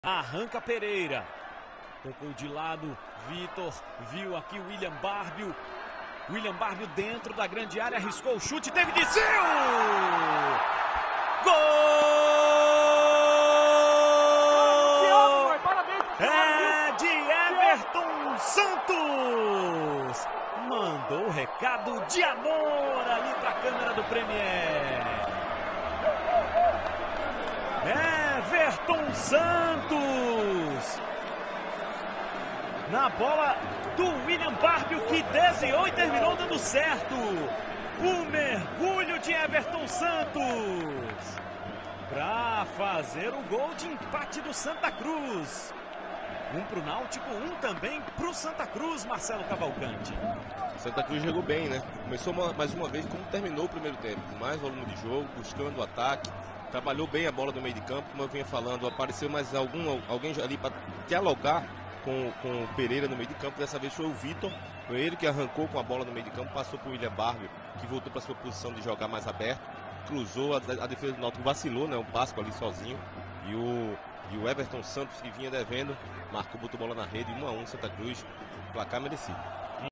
GOL - Éverton Santos - Santa Cruz x Náutico [10/04/2017] (0000)
Após um chute do Willian Barbio que desviou no marcador e sobrou para o Éverton Santos dentro da área que aproveitou para marcar o gol de cabeça.